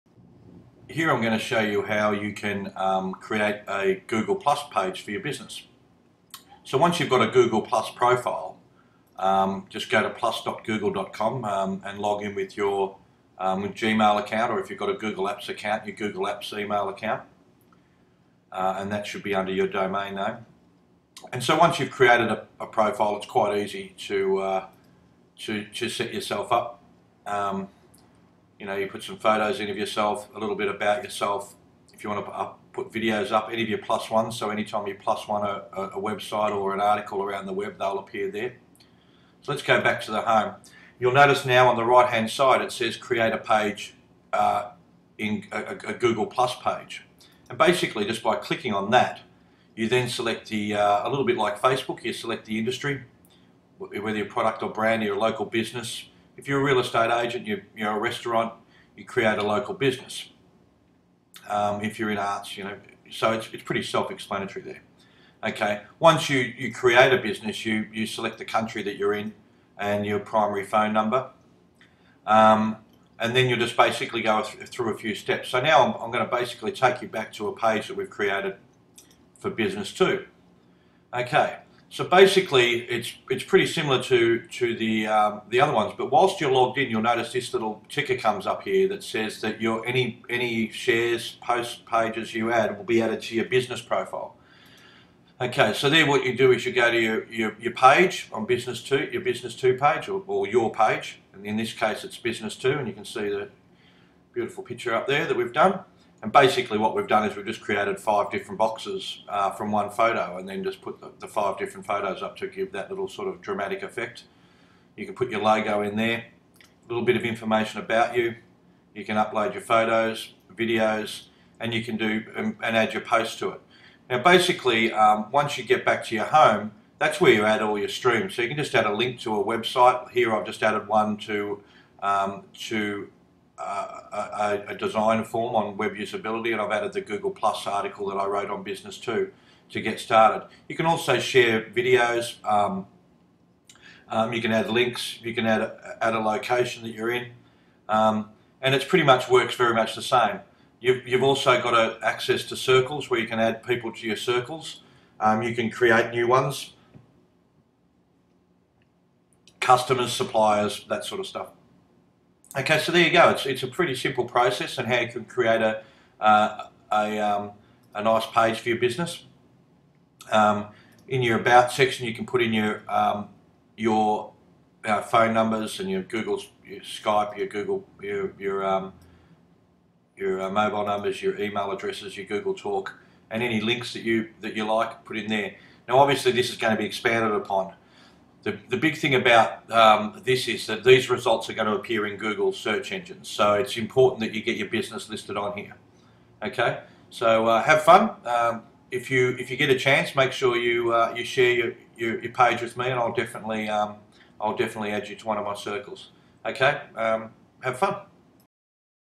Google Plus For Business
Here is a little primer on how to create a page on Google+